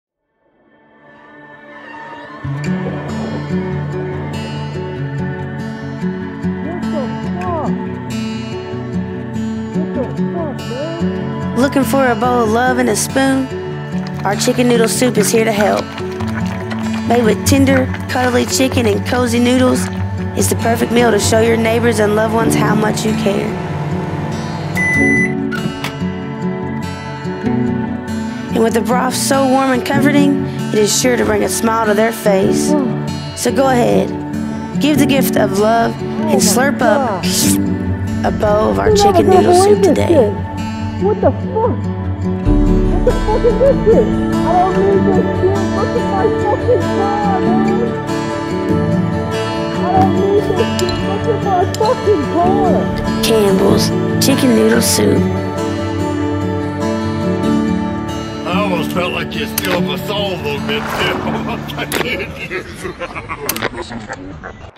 Banned Super Bowl Commercial - Campbell's Chicken Noodle Soup Commercial
Banned Super Bowl Commercial - Campbell's Chicken Noodle Soup Commercial Skit

Improved during the snow storm a TV Head Original.